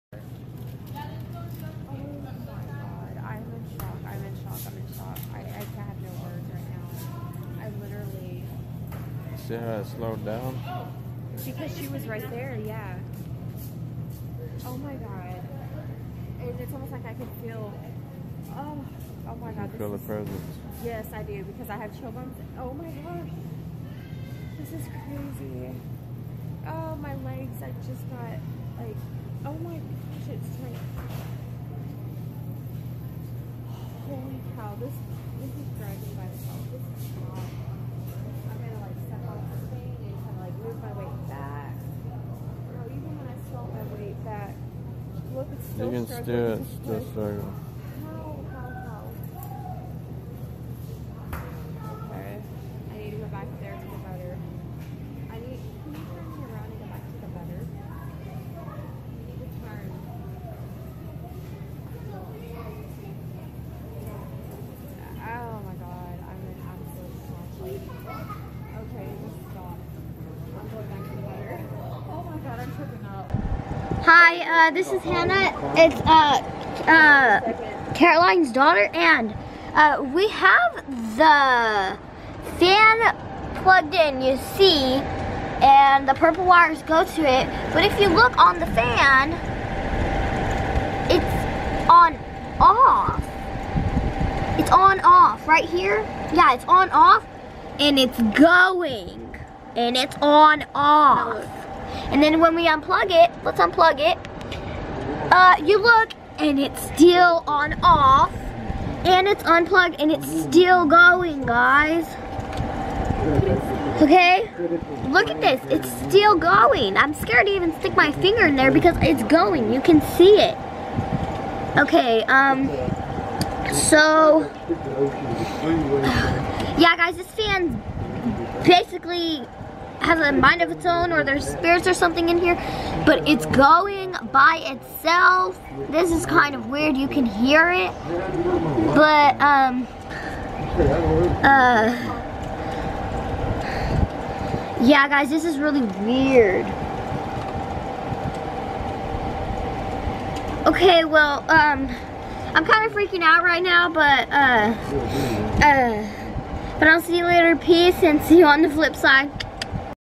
PROOF THE PARANORMAL FOLLOWS ME!!! MUST WATCH
I FILMED THESE TWO VIDEOS WITH MY CAMERA, AND THEY WERE FILMED ONLY A FEW DAYS APART RECENTLY, AND YOU CAN CLEARLY SEE THAT WHILE I AM SHOPPING IN WALMART THE CART I AM RIDING IN STARTS DRIVING ITSELF THE SWITCH TO TURN THE CART OVER IS HEAVY N HARD TO PUSH SO IT WAS A STRONG FORCE TO BE ABLE TO PUSH THE FORWARD BUTTON AND STEER AND MY WEIGHT ON TOP OF IT ALL. THE FAN IN MY BEDROOM THAT YALL SEE I ALWAYS USE IN MY MAKEUP TUTORIALS IS THIS SAME FAN MY DAUGHTER RECORDED STARTING GOING OFF IN THE OFF POSITION, AND SO WE CHECKED IT OUT N  U CAN SEE HER UNPLUG IT FROM THE OUTLET N IT IS STILL ON THE OFF BUTTON AND GOING AT THE HIGHSPEED WHILE UNPLUGGED. EVER SINCE I WAS A LIL GURL I HAD ALWAYS FELT AS IF I WAS BEING WATCHED, AND I WOULD HEAR AND SEE THINGS WHATEVER FOLLOWS ME AROUND HAS NEVER LEFT IDK IF ITS DIFF SPIRITS ANGELS DEMONS OR THE SAME ONES BUT IM CURRENTLY 33 AND SINCE I WAS REAL YOUNG PARANORMAL THINGS HAVE BEEN HAPPENING TO ME IM SO USED TO IT BY NOW BUT I THOUGHT IT WOULD BE KEWL TO SHARE THESE TWO THINGS OCCURING ON MY CAM I CAUGHT.  PEACE ILL SEE  U ON THE FLIP SIDE.